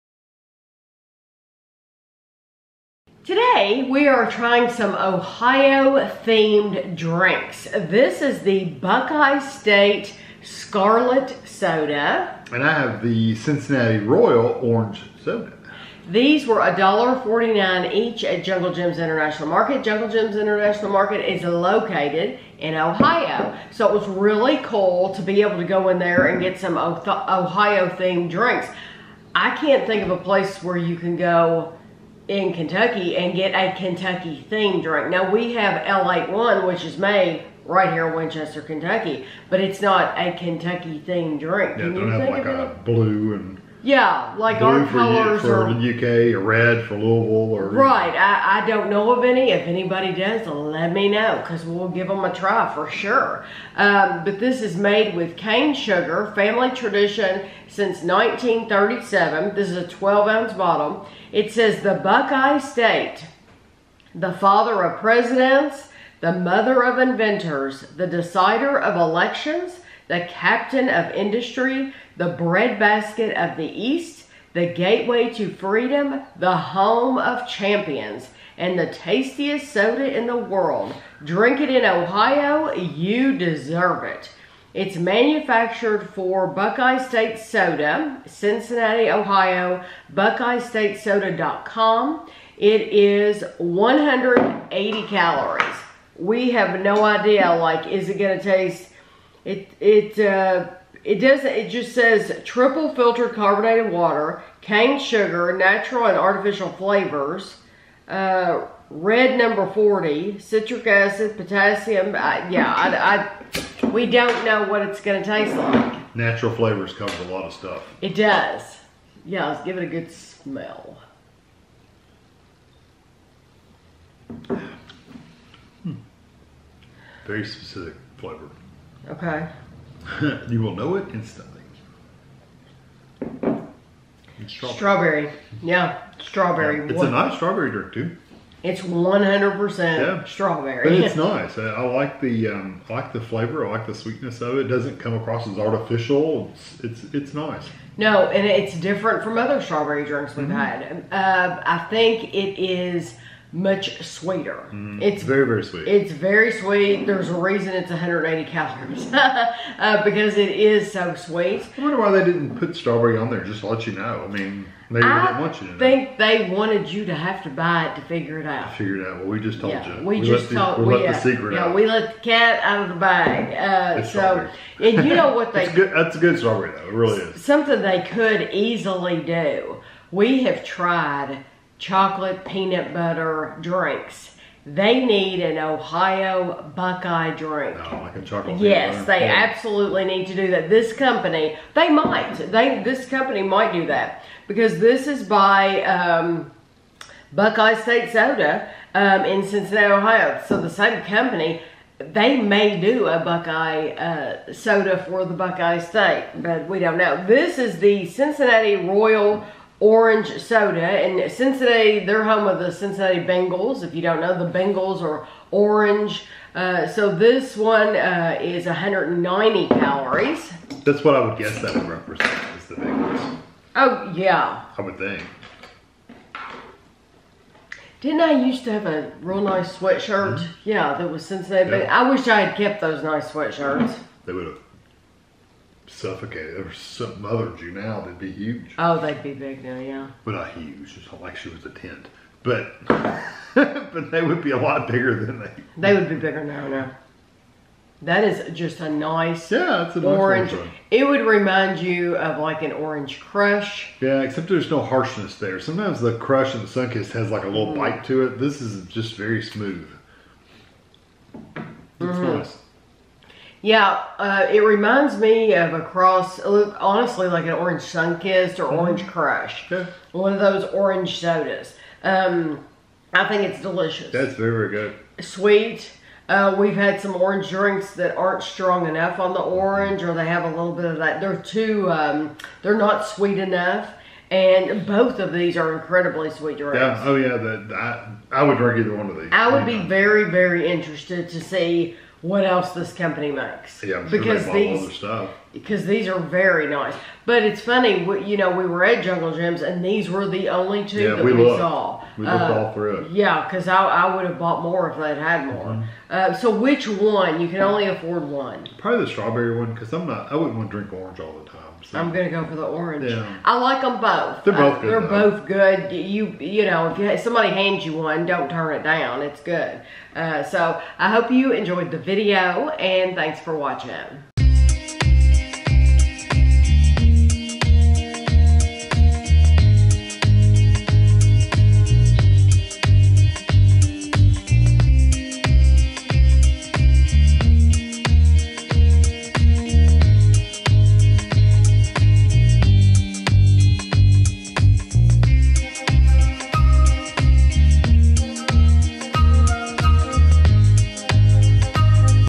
Buckeye State Scarlet Soda & Cincinnati Royal Orange Soda Review
This is a taste test/review of the Buckeye State Scarlet Soda and the Cincinnati Royal Orange Soda. They were bought at Jungle Jim’s International Market.